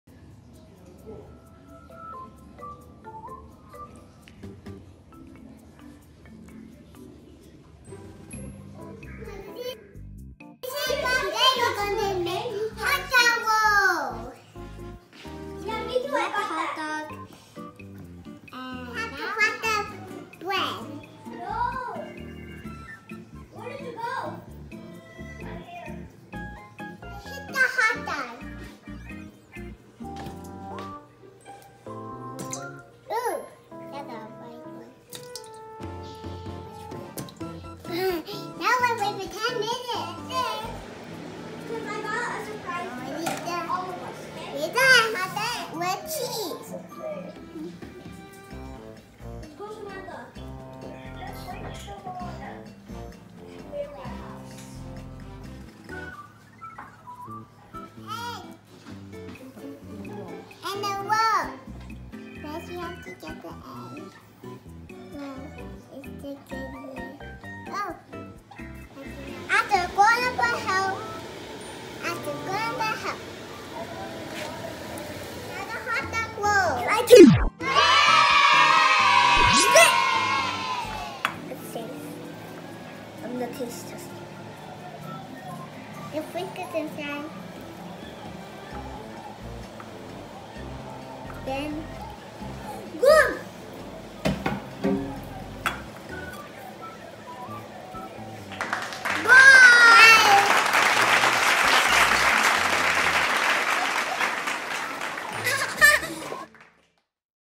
Hotdog Roll | Little Chef
Recipe for hotdog roll by little chef
1.bread
2.bread crumbs
3.hotdog
4.cheese
 watch the whole video for the cooking instruction.